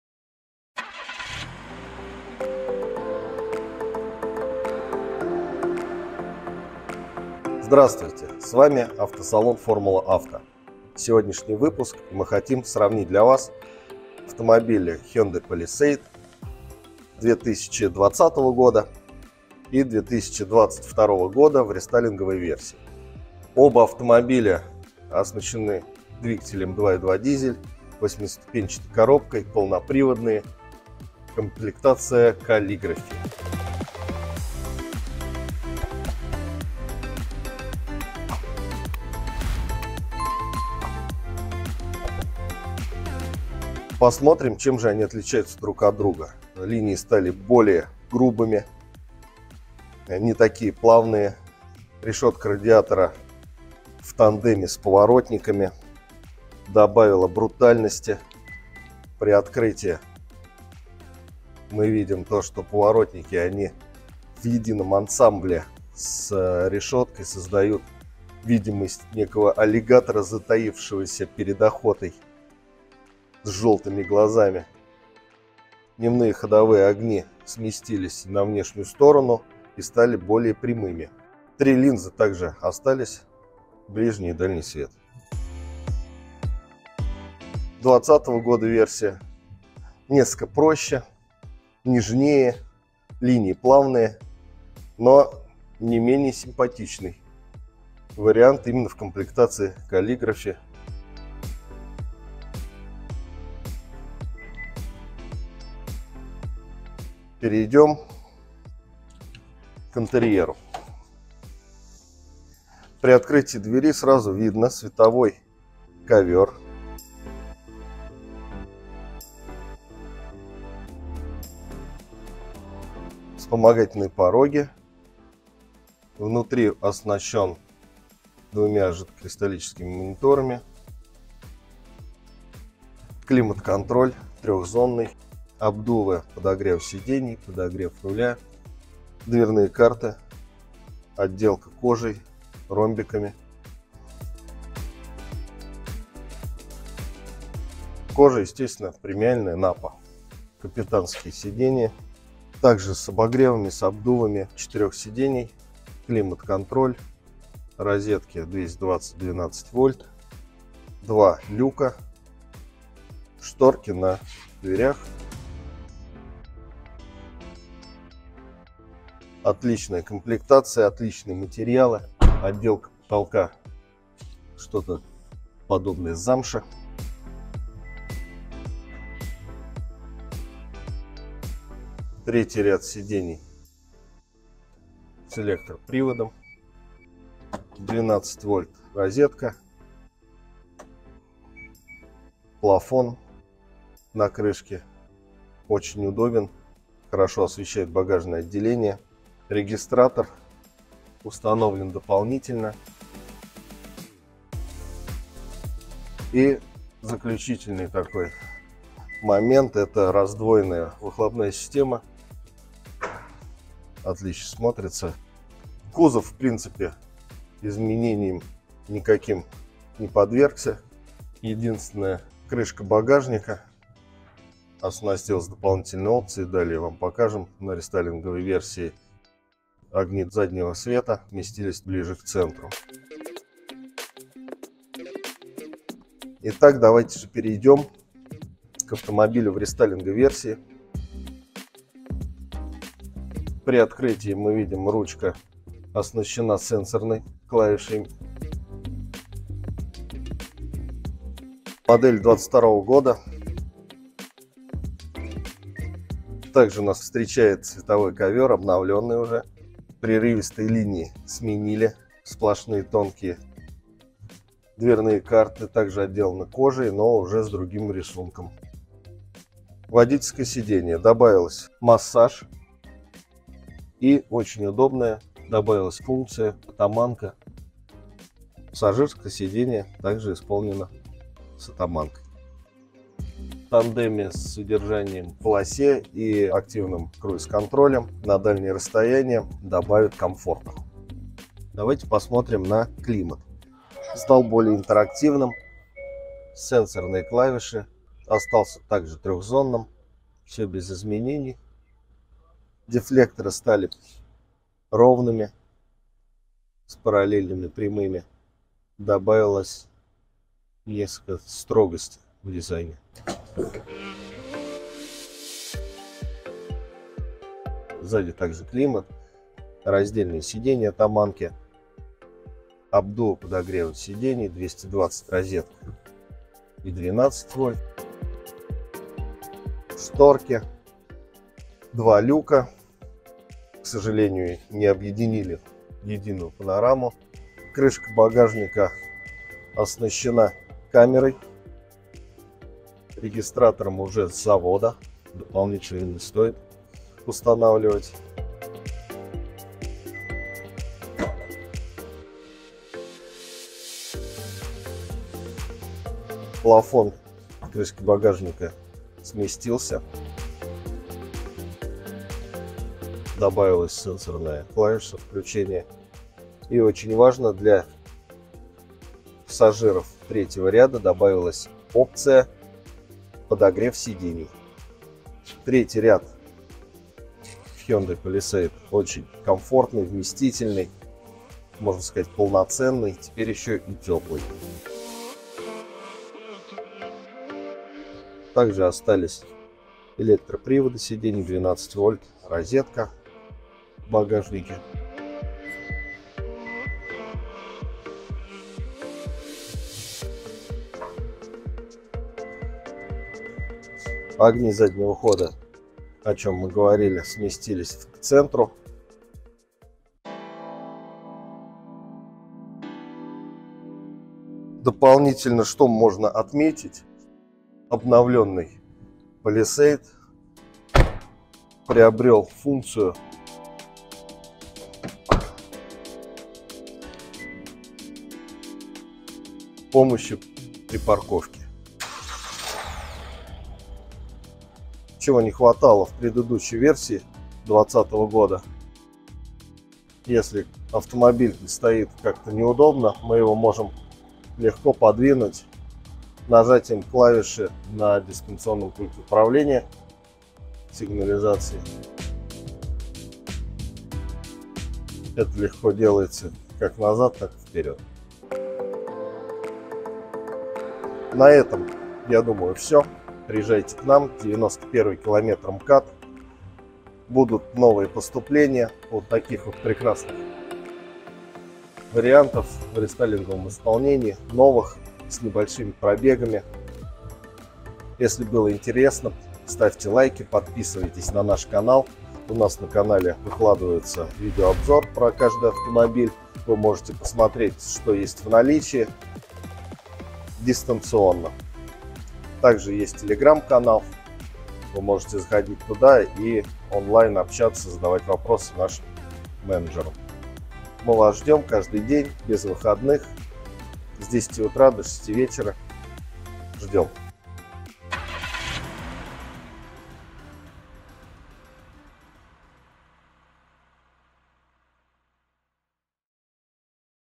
Обзор Hyundai Palisade Calligraphy Рестайлинг 2022г сравнение с Hyundai Palisade Calligraphy 2020г
Обзор и сравнение дизельных Hyundai Palisade Calligraphy Рестайлинг 2022г и Hyundai Palisade Calligraphy дорестайлинг. Большой выбор в Москве Hyundai Palisade, Hyundai Staria, Hyundai Tucson, Kia Mohave, Kia Sportage, Kia Carnival, Kia Sorento. , а так же любые авто из Кореи.